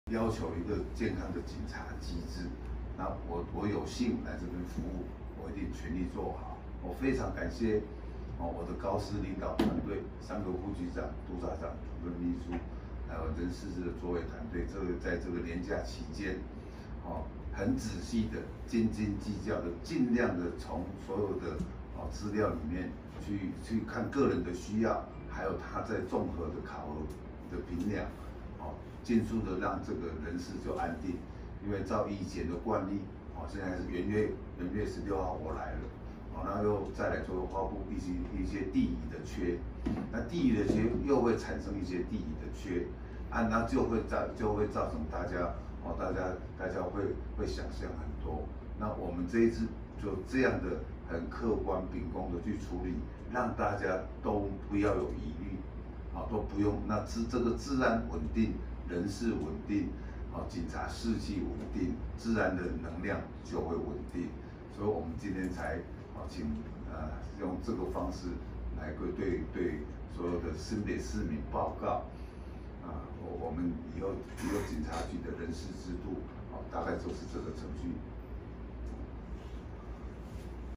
開春第一天 新北市警察局發佈人事異動
記者蔡琇惠／新北報導

為應現階段治安工作勤、業務需要，配合各序列職缺檢討陞職，新北市警察局17日發佈新一波人事異動，市警局表示，經依相關適任人員之品操學能、工作績效、職務歷練及職期調任等原則通盤檢討，並尊重各單位主官（管）考核用人意見、兼顧同仁服務年資及自請調地意願進行職務調整，期使各員適才適所，發揮所長，提升整體治安維護效能。

市警局長黃宗仁表示，相關人事調整完全是貫徹侯市長在布達典禮上再三強調的警察人事一條鞭，避免關說，其實也就是希望大家都按照規定，專心做好治安及交通本務！黃宗仁強調，未來所有的新北市警察同仁有任何異動上的想法，都可以依程序寫報告表示，並依【資績、年資、主官管考核】及同仁的權益來思考如何做【適切】調整。

市警局指出，相關異動人員除第九序列非主管職務調陞第八序列非主管職務人員（計4員）另案辦理公開選填作業，報到日期另訂外，其餘人員（計121員）統一律定於2月19日15時前至新職單位報到。